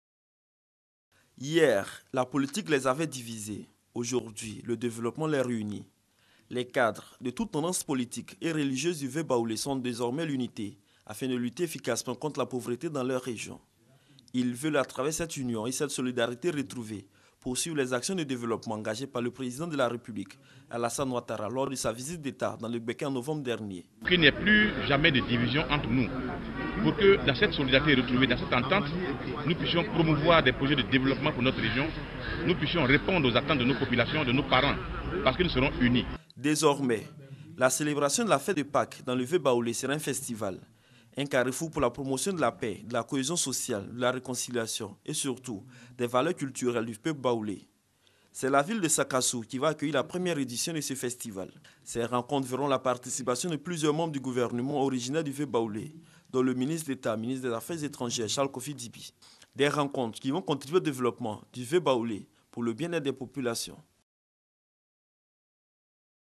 Les cadres du "V" baoulé (centre de la Côte d'Ivoire) s'unissent pour un développement local
Les cadres du "V" baoulé (centre de la Côte d'Ivoire) s'unissent pour le développement